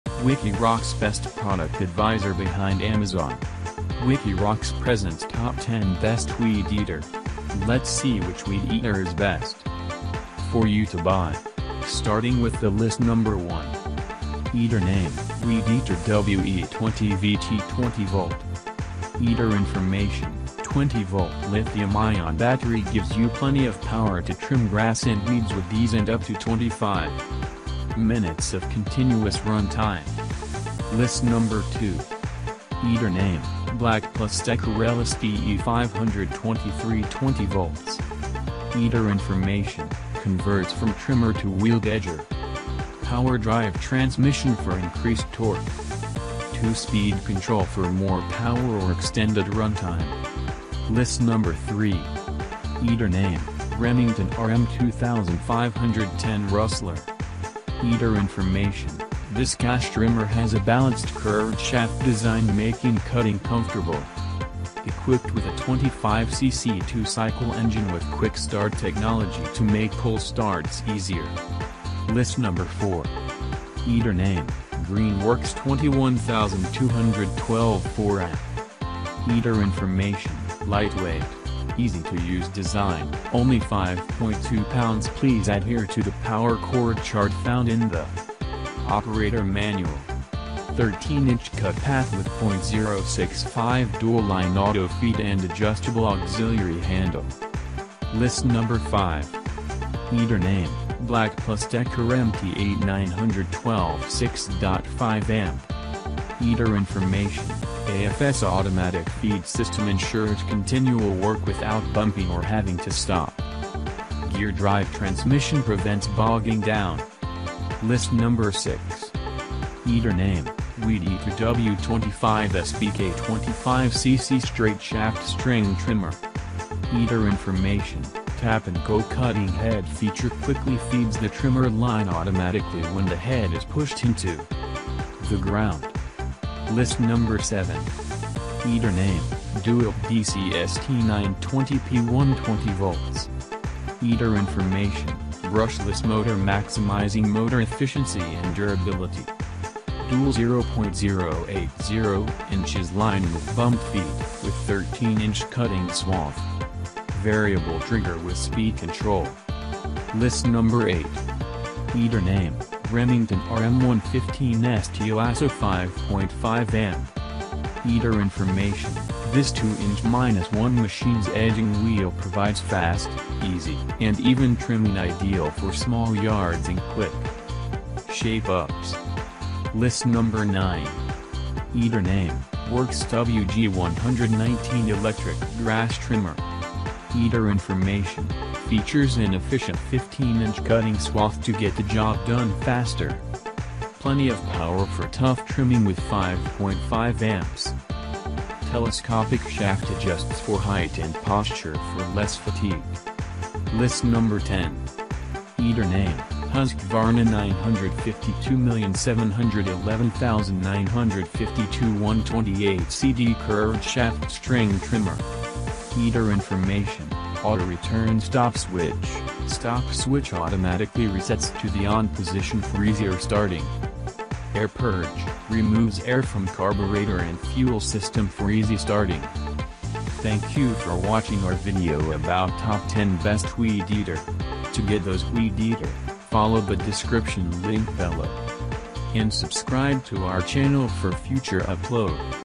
Top 10 Best weed eater
This video is about Top 10 Best weed eater you will get best idea about  weed eater.

Eater name: Weed Eater WE20VT 20-Volt .
Eater information: 20-Volt Lithium-ion battery gives you plenty of power to trim grass and weeds with ease and up to 25 minutes of continuous run time.

List number 2,
Eater name: BLACK+DECKER LSTE523 20V.
Eater information: Converts from trimmer to wheeled edger.
Power Drive Transmission for increased torque.
2-speed control for more power or extended runtime.

List number 3,
Eater name: Remington RM2510 Rustler.
Eater information: This gas trimmer has a balanced curved shaft design making cutting comfortable.
Equipped with a 25cc 2-cycle engine with QuickStart technology to make pull starts easier.

List number 4,
Eater name: GreenWorks 21212 4Amp.
Eater information: Lightweight, easy to use design, only 5.2 lbs. Please adhere to the power cord chart found in the operator manual. 13-inch cut path with .065 dual line auto-feed and adjustable auxiliary handle.

List number 5,
Eater name: BLACK+DECKER MTE912 6.5-Amp.
Eater information: AFS automatic feed system ensures continual work without bumping or having to stop.
Gear Drive transmission prevents bogging down.

List number 6,
Eater name: Weed Eater W25SBK 25cc Straight Shaft String Trimmer.
Eater information: Tap 'N Go Cutting Head feature quickly feeds the trimmer line automatically when the head is pushed into the ground.

List number 7,
Eater name: DEWALT DCST920P1 20V.
Eater information: Brushless Motor maximizing motor efficiency and durability.
Dual 0.080" line with bump feed, with 13-inch cutting swath.
Variable trigger with speed control.

List number 8,
Eater name: Remington RM115ST Lasso 5.5 Amp.
Eater information: This 2-in-1 machine's edging wheel provides fast, easy, and even trimming ideal for small yards and quick shape-ups.

List number 9,
Eater name: WORX WG119 Electric Grass Trimmer.
Eater information: Features an efficient 15-inch cutting swath to get the job done faster.
Plenty of power for tough trimming with 5.5 Amps.
Telescopic shaft adjusts for height and posture for less fatigue.

List number 10,
Eater name: Husqvarna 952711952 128CD Curved Shaft String Trimmer.
Eater information: Auto Return Stop Switch - Stop switch automatically resets to the ON position for easier starting.
Air Purge - Removes air from carburetor and fuel system for easy starting.